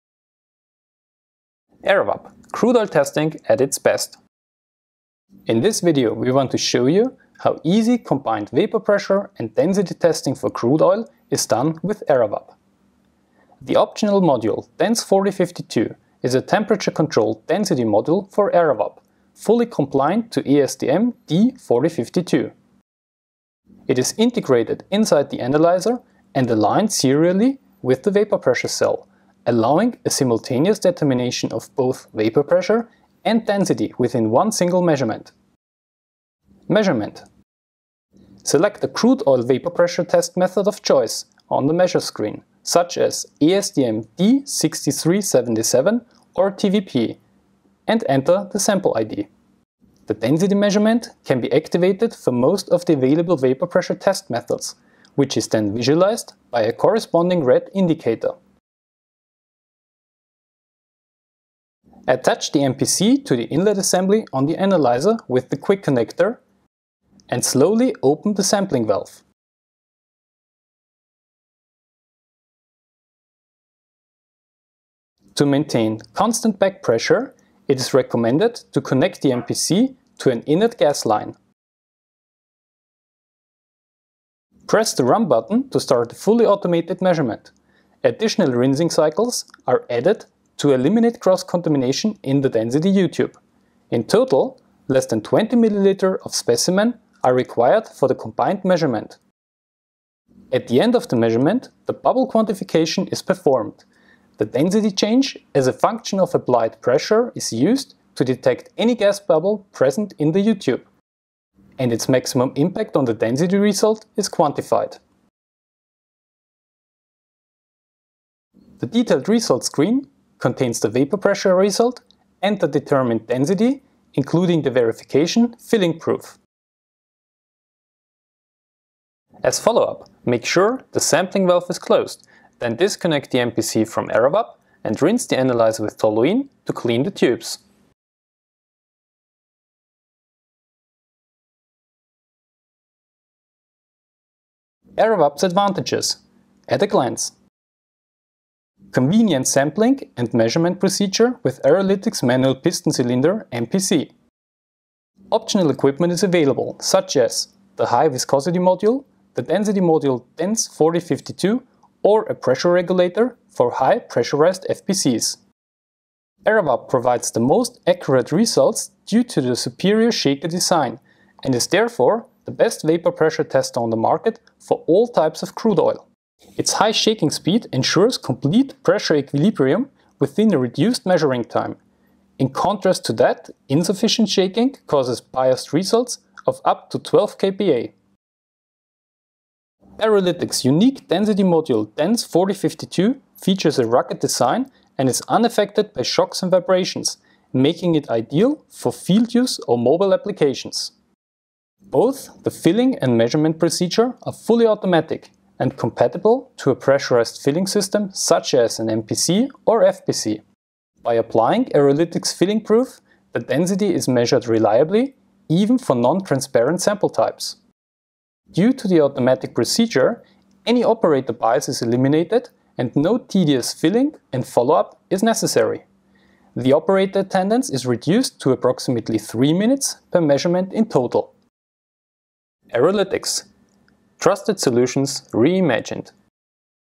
Application Video: ERAVAP - Crude Oil Testing at its Best
Watch eralytics latest application video and convince yourself how we set the benchmark for Vapor Pressure + Density testing of crude oil combining ERAVAP with eralytics revolutionary density meter module DENS4052 and our new piston cylinder ERAVAP MPC.